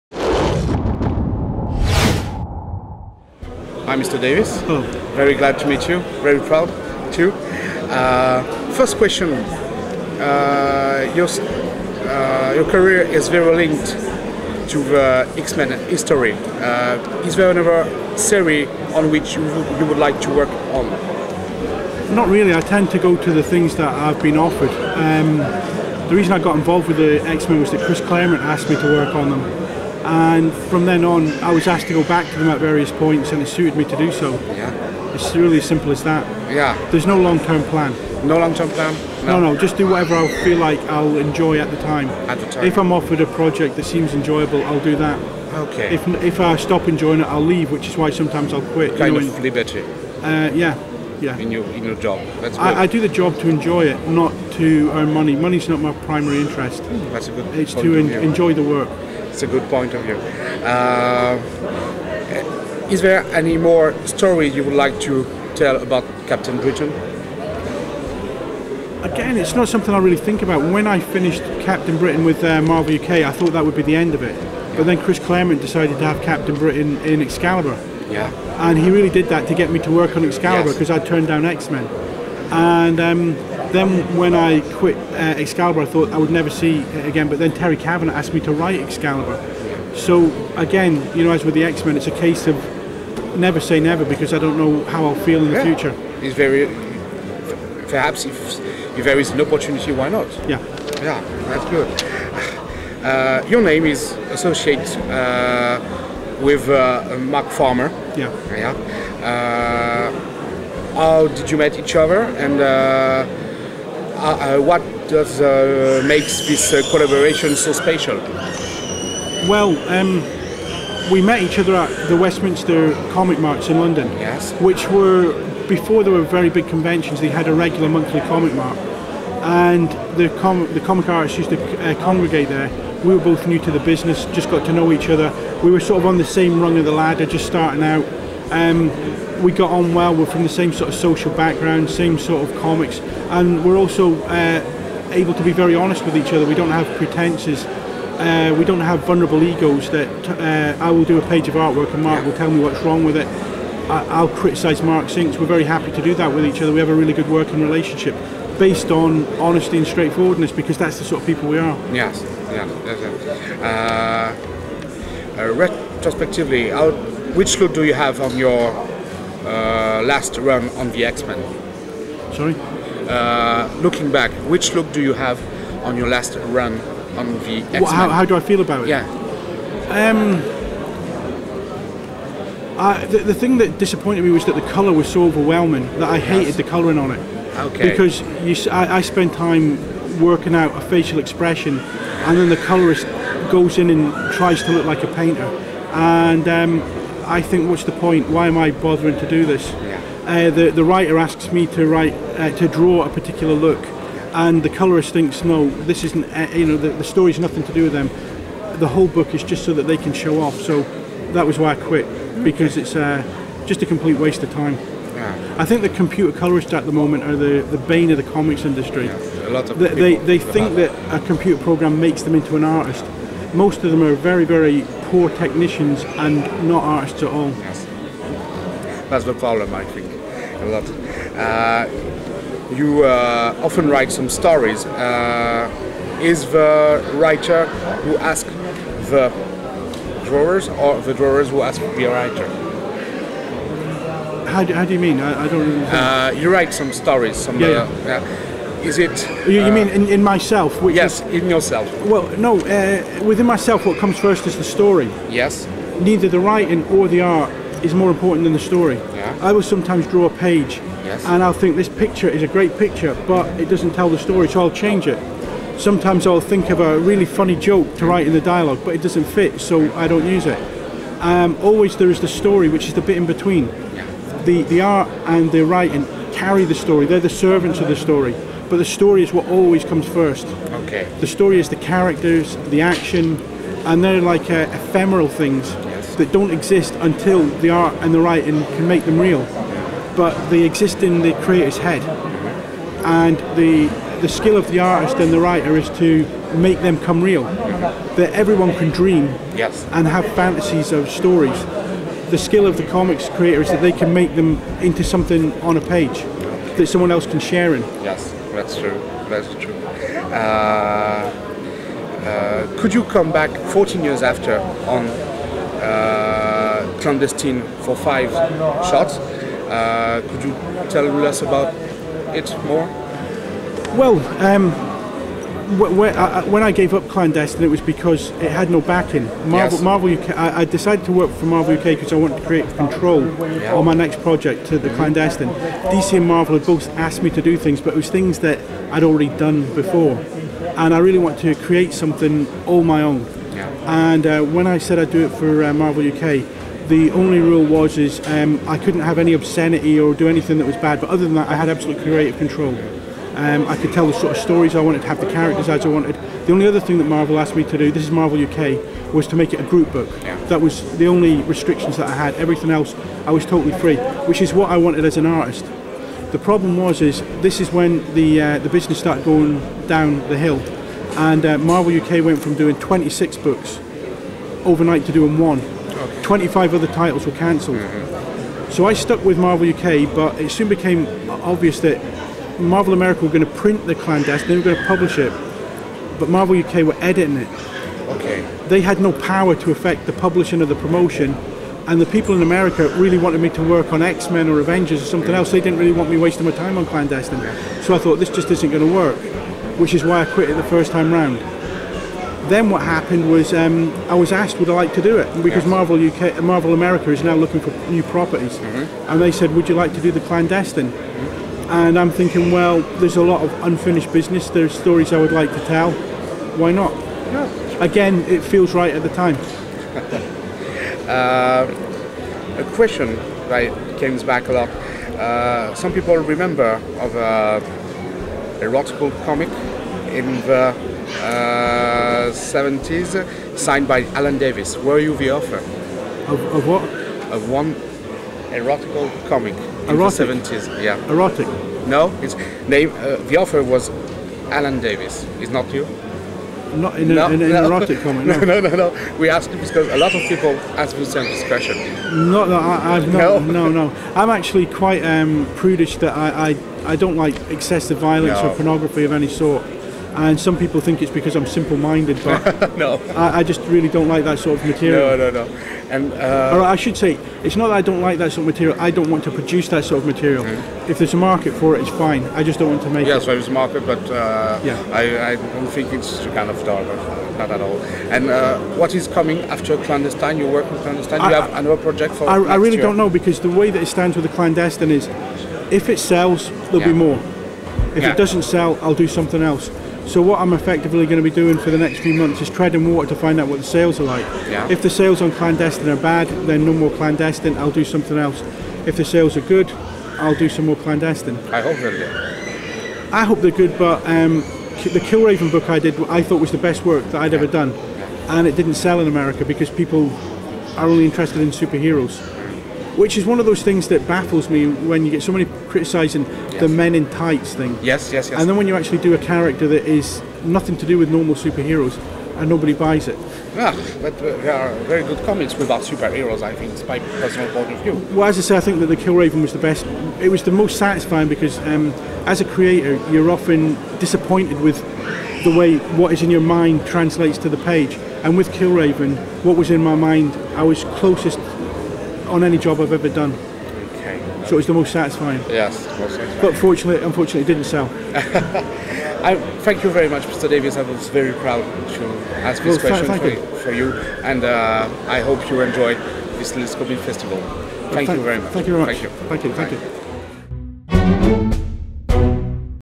Lille comics festival - Entretien avec Alan Davis
Interview d'Alan Davis réalisée par le Lille Comics Festival en 2007